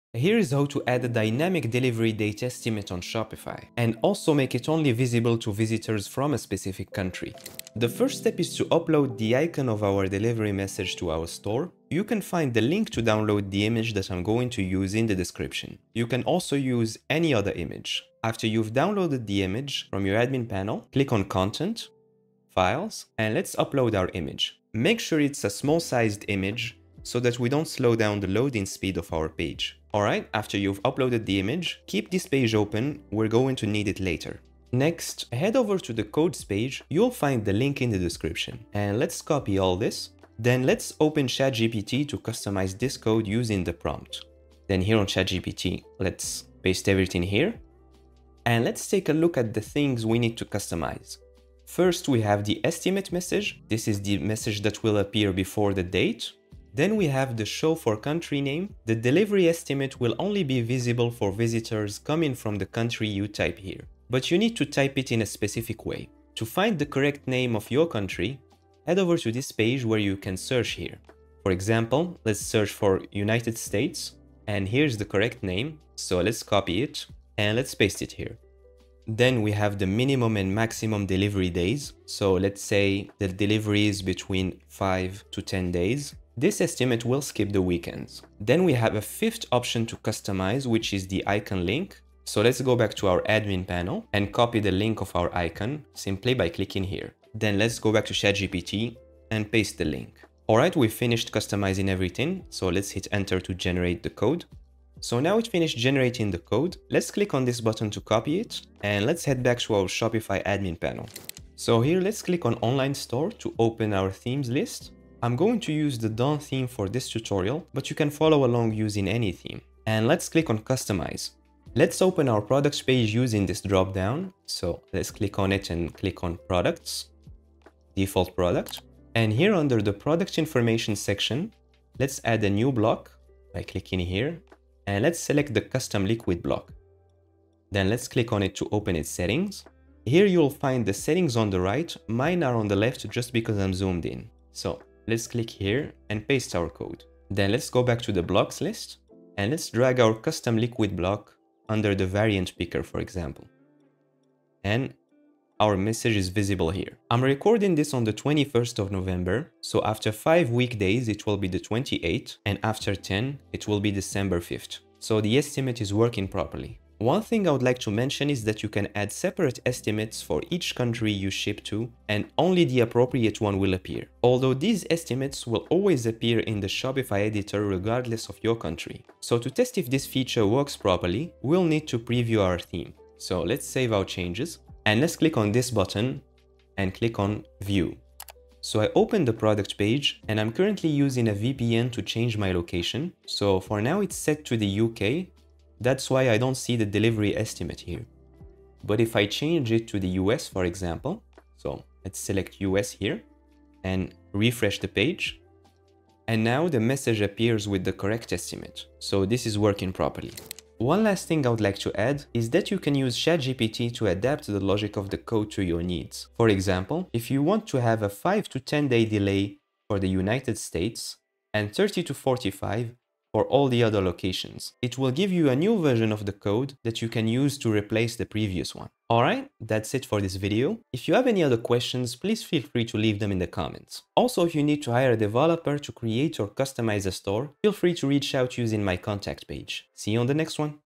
Estimated Delivery Dates Based on Visitor's Country in Shopify
Show a delivery date estimate based on your visitor's country with this easy-to-follow Shopify tutorial! Learn how to add a delivery range date and implement location detection to display country-specific shipping details. This method works seamlessly with any Shopify theme, including popular ones like Dawn, making it perfect for stores looking to provide accurate delivery estimates and improve shipping transparency. Whether you want to boost customer trust, optimize your shipping process, or enhance your store’s user experience, this guide has you covered with practical steps for any Shopify store.